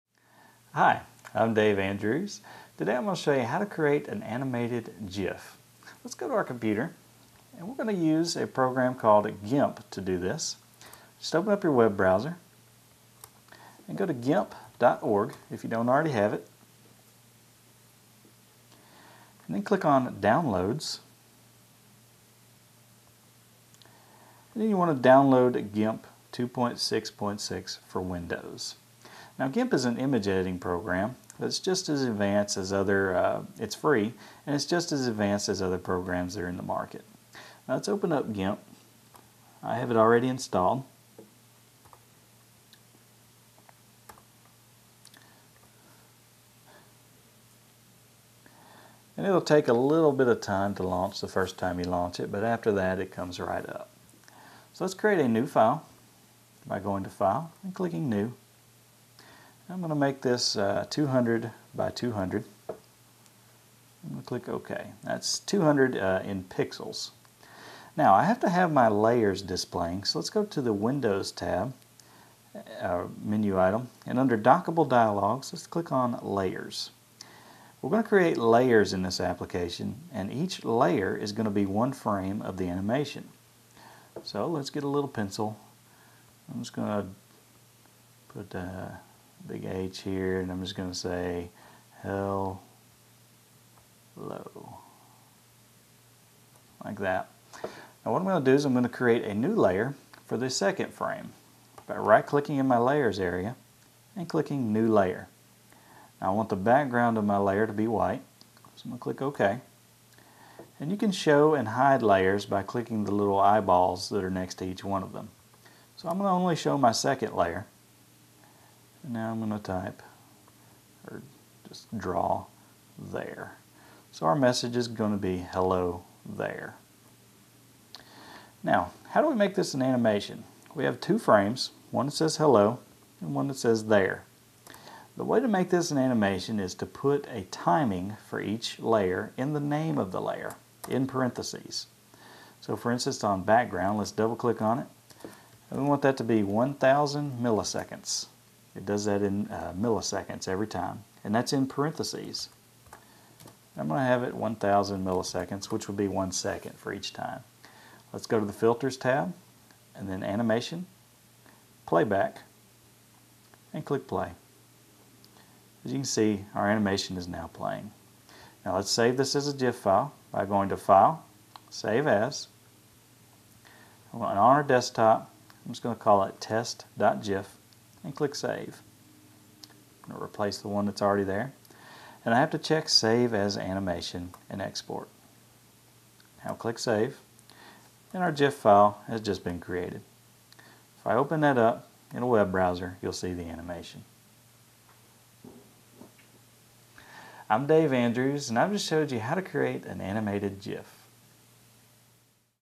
How to Create an Animated GIF
Creating an animated GIF is best done using a free image-editing program called GIMP, where each frame can be illustrated in each layer. See how simple animations are possible in GIF format with helpful tips from a software developer in this free video on computers.